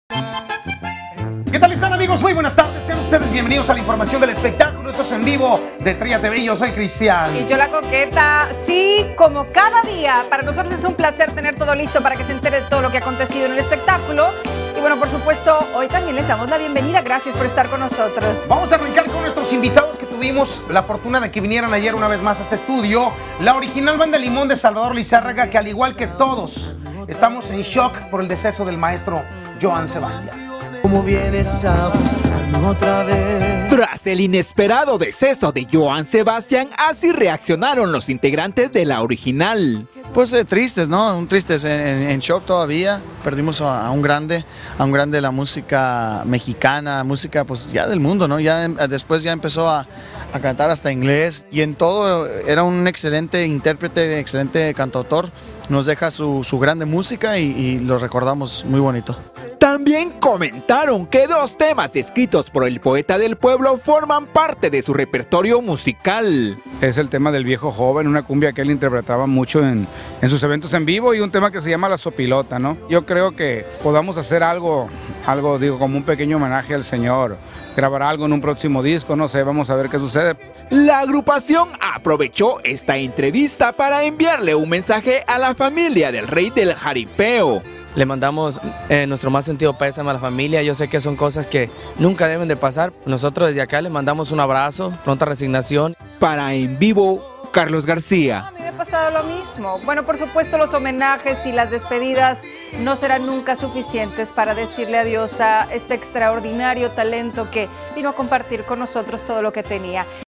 La Original Banda Limon en shock En Vivo
Original Banda Limon en shock por la muerte de Joan Sebastian.